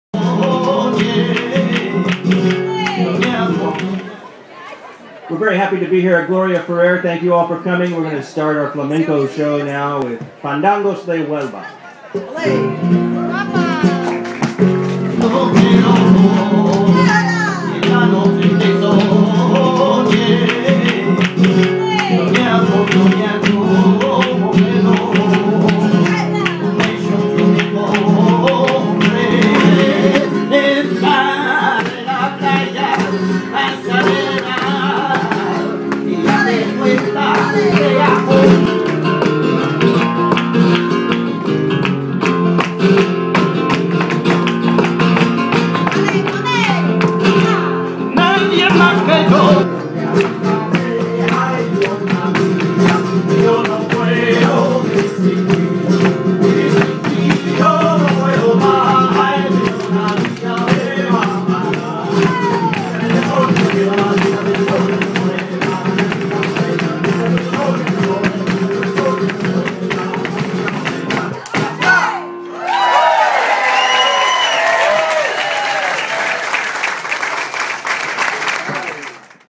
Catalan Festival.mov
Flamenco dancers perform at the 2011 Catalan Festival, celebrating the 25th anniversary of Gloria Ferrer.